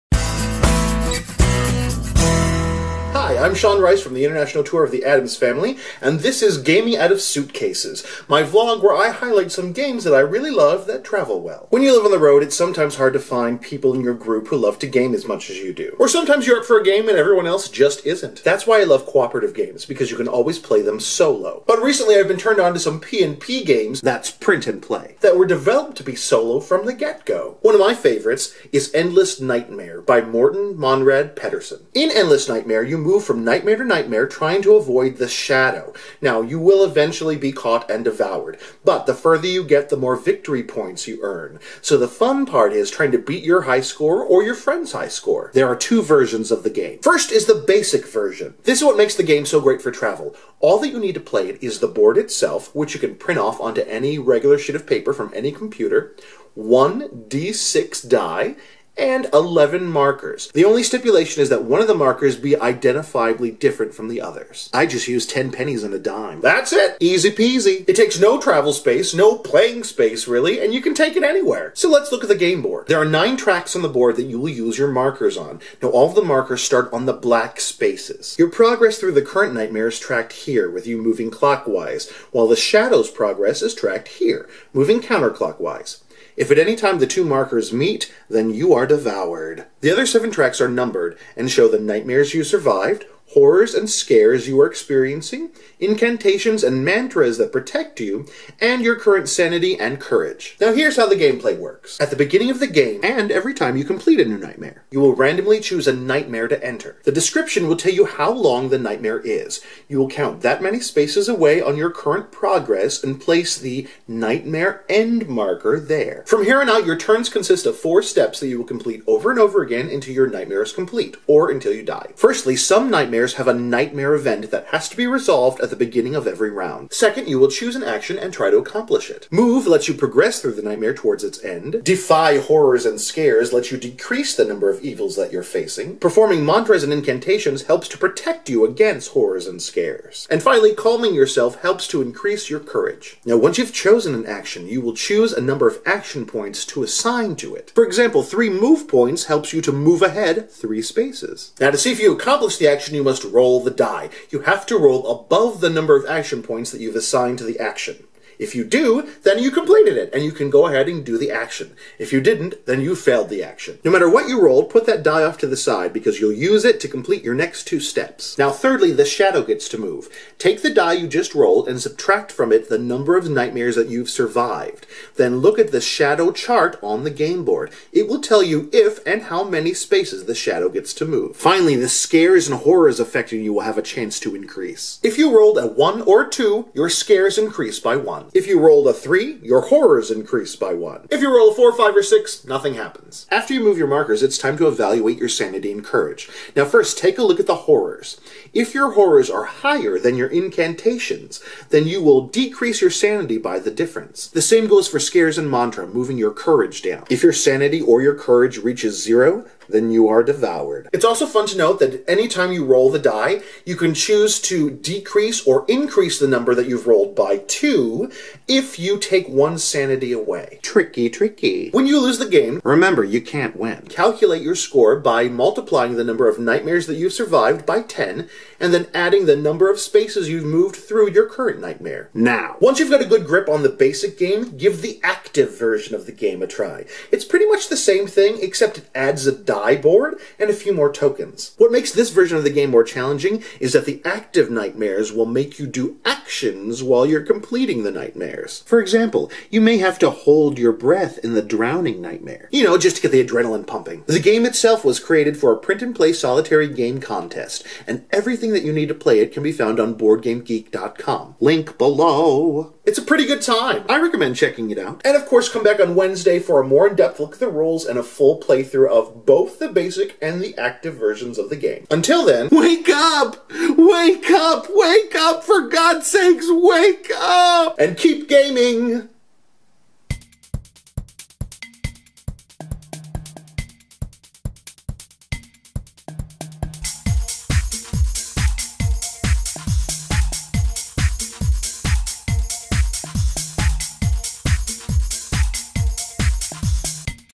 Endless Nightmare
Hosted by: Shaun Rice

Game of the Week:
Endless Nightmare by Morten Monrad Pederson

Upcoming Tour Dates:
4/8-13   Columbus, OH